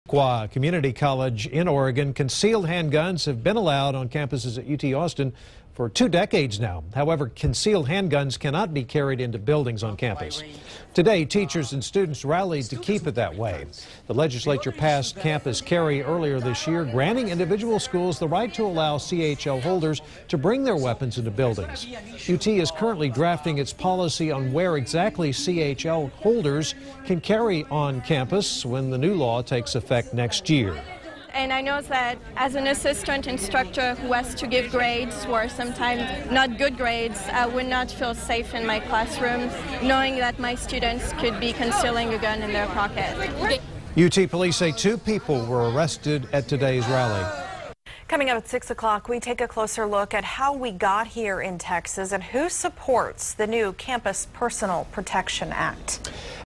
Gun Free UT Rally on campus
Two people were arrested when they trespassed on the rally.